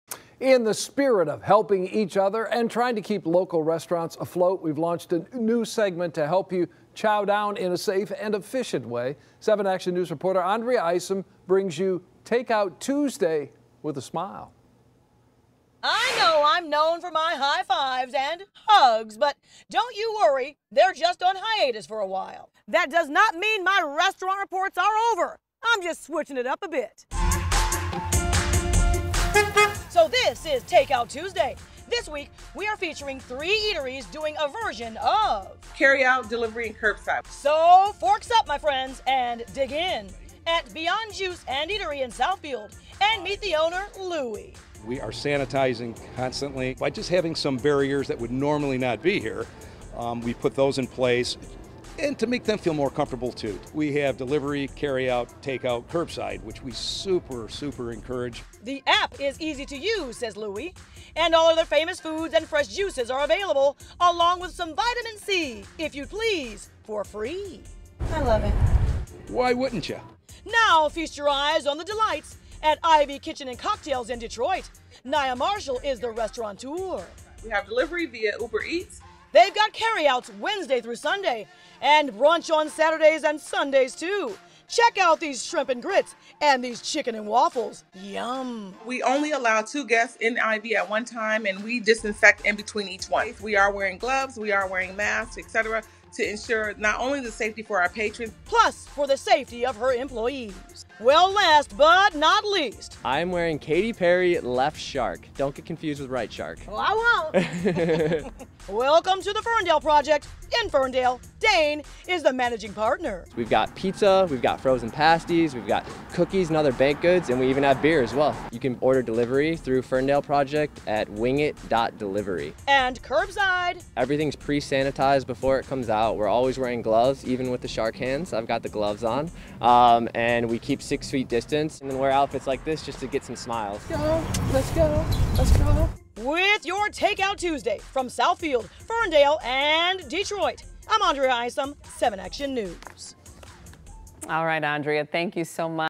We're Open Detroit: Take Out Tuesday!
In the spirit of helping each other and trying to help our communities, friends, and our neighbors in the restaurant business stay afloat, we have launched a new segment to help you, “chow down” in a safe and efficient way for you and your families.